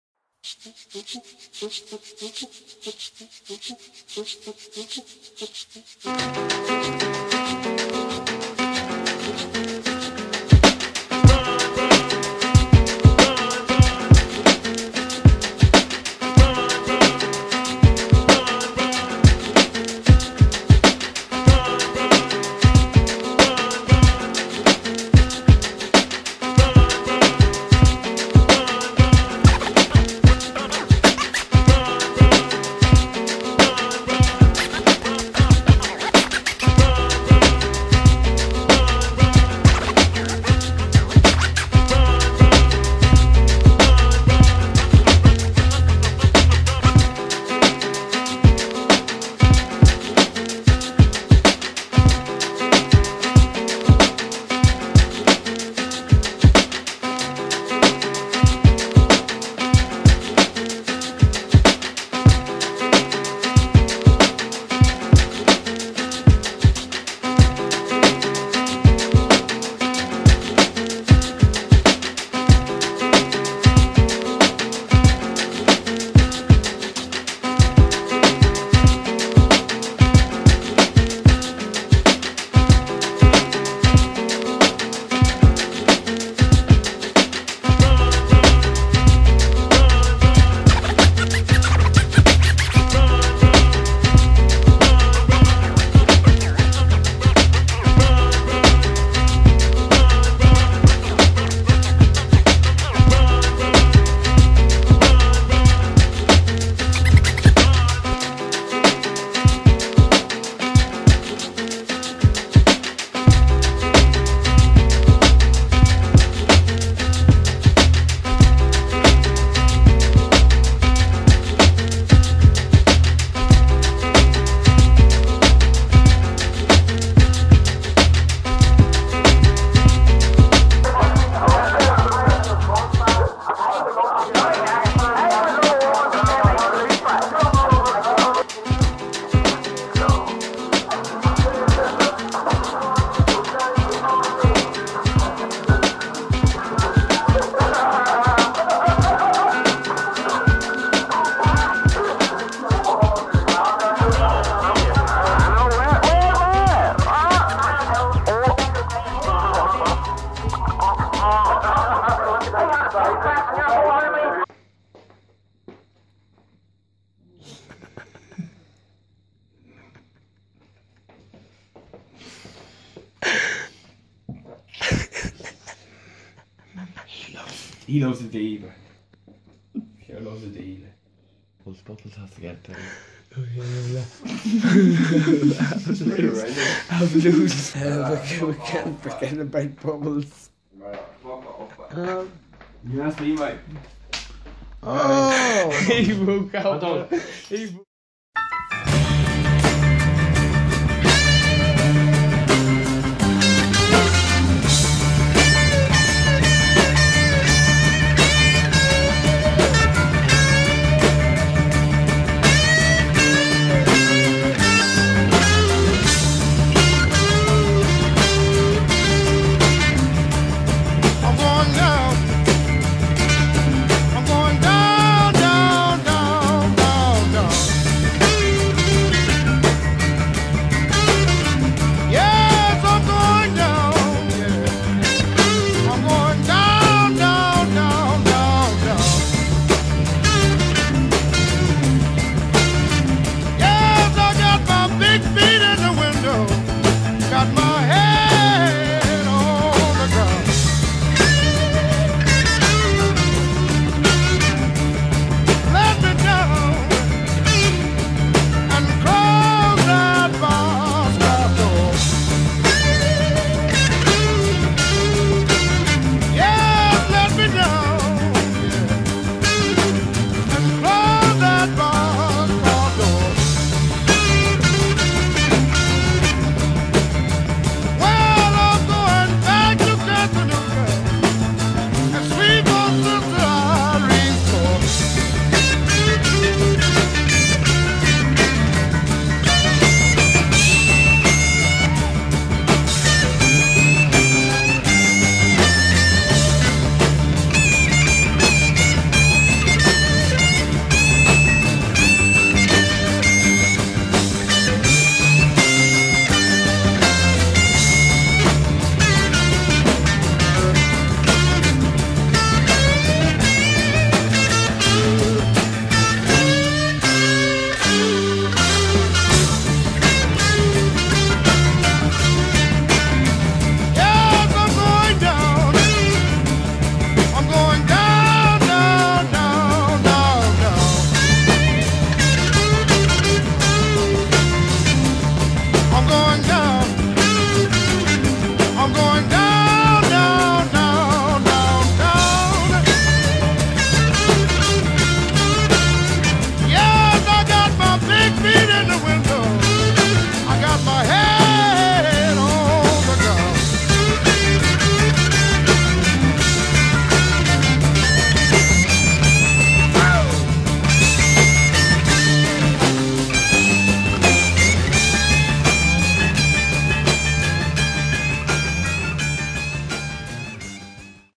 Going down - skateboarding
Some skateboarding in Newry and T13 plus party craic!